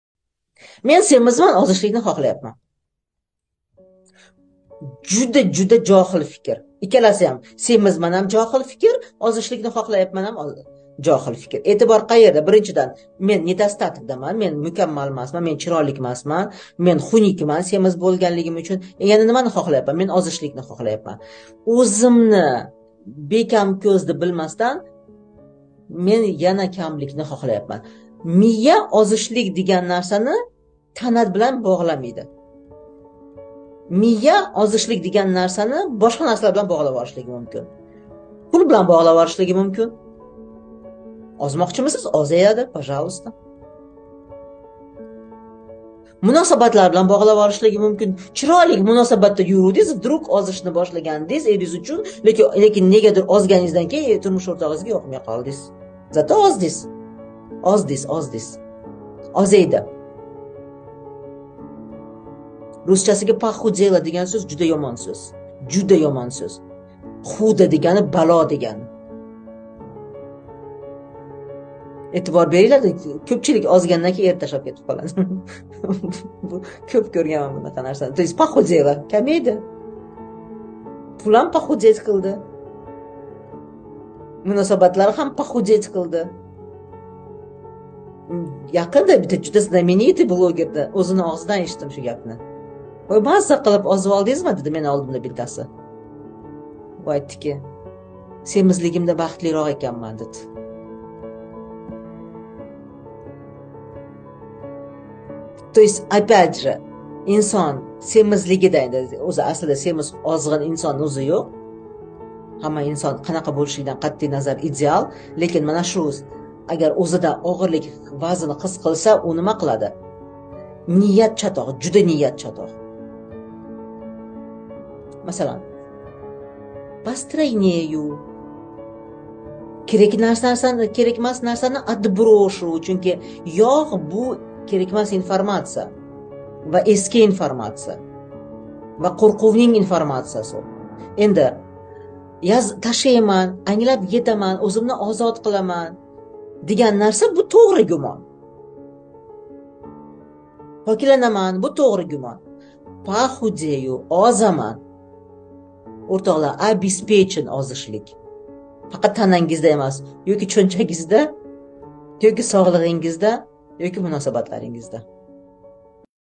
Semizman ozishlikni xohlayman....
Kundalik fikrlardagi jaholat.
Maqsadlaringizni qanday niyat va taxmin bilan belgilaysiz?
Istaklaringizga erishishda so'zlarni to'g'ri tanlaysizmi? Bunga
e'tibor bering.
Ekspertlik kursining dars tahlilidan parcha.